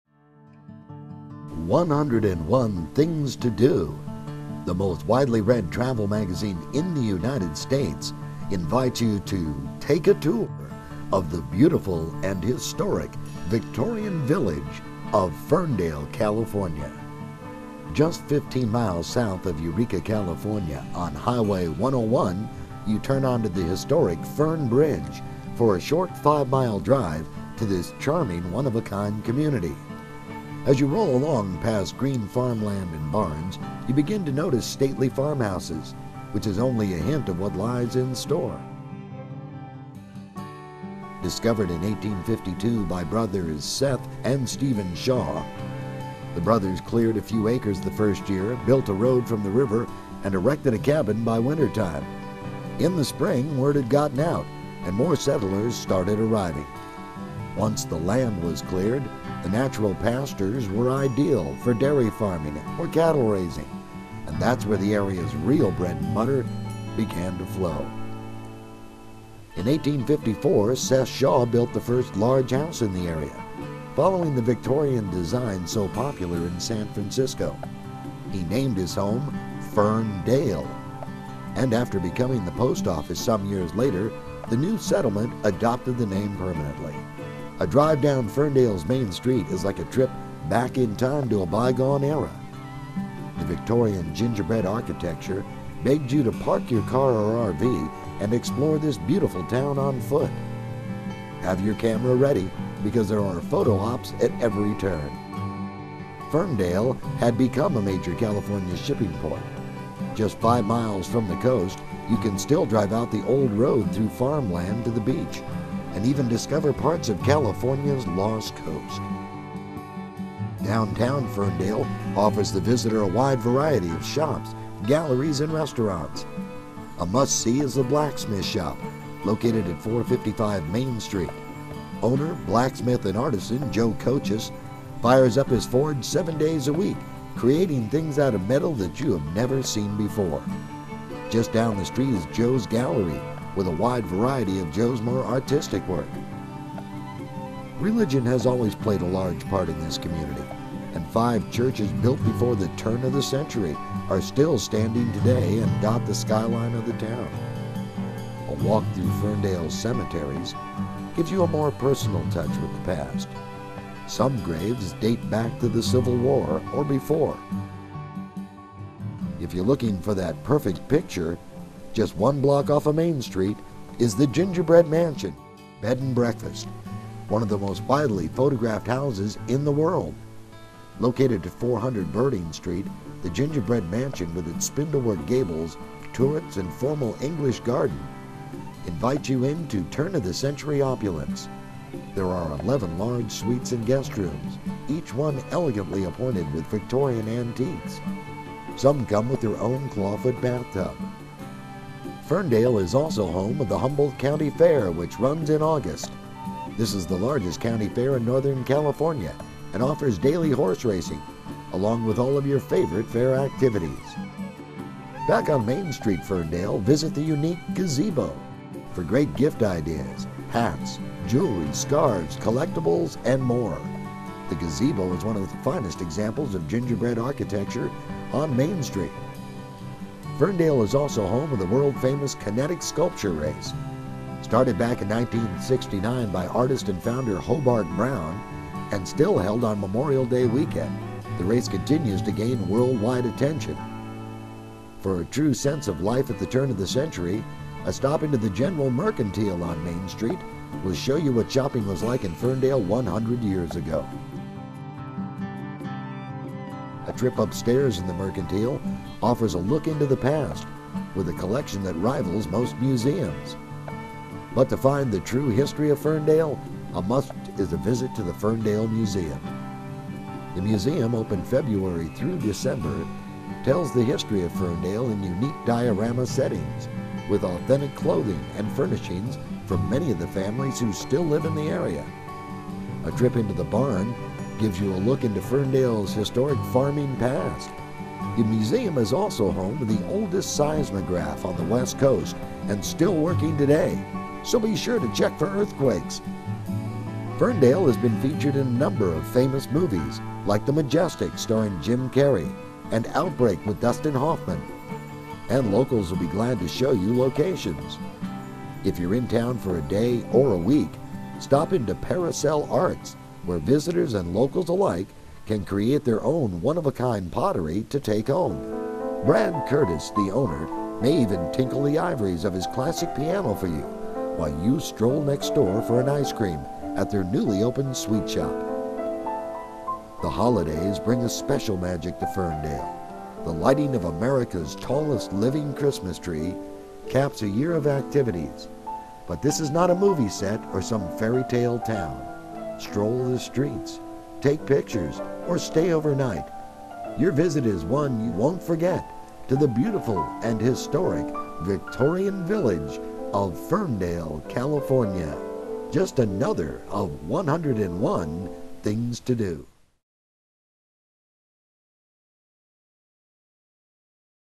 Ferndale, CA
Various things to see and do in the Historic Victorian Village of Ferndale, CA.